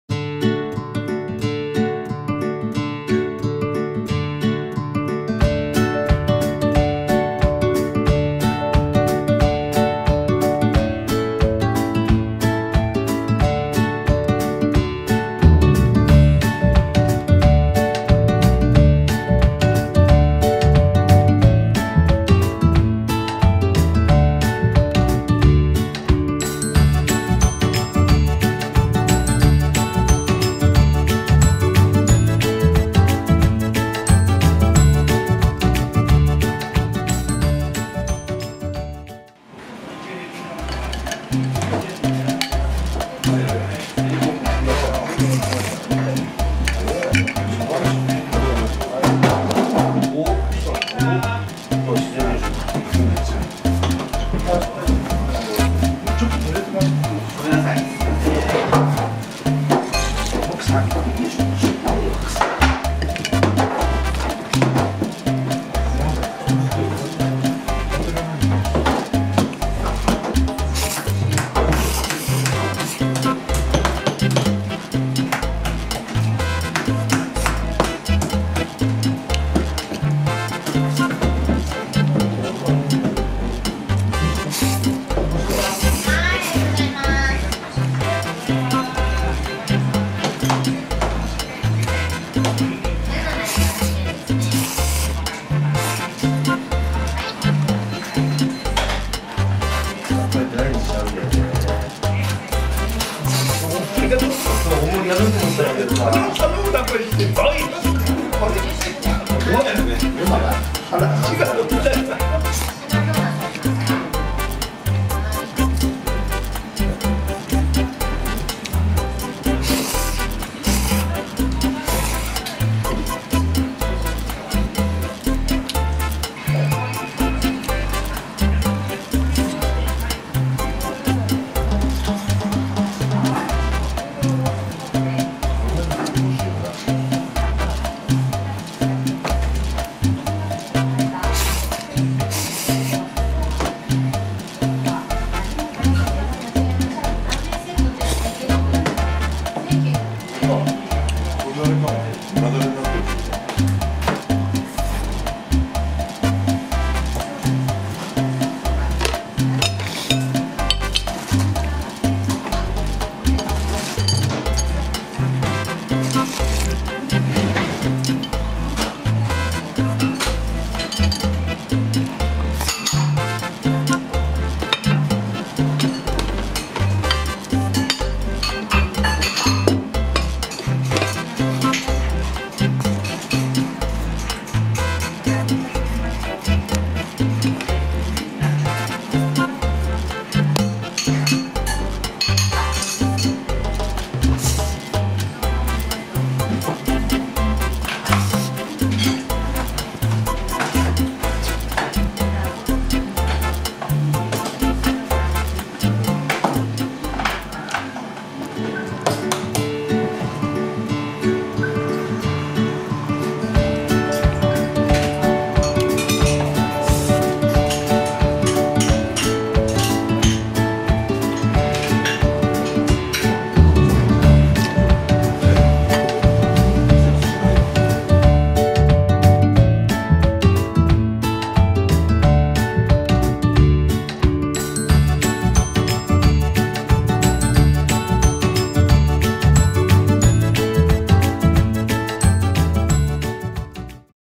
千葉市で竹岡式 つち家 竹岡氏を訪ねて 【飯動画】
千葉市に珍しい竹岡式ラーメンに行ってきました。欲張ってセットメニューに手を出してしまいました。　近くにあるって良いですね。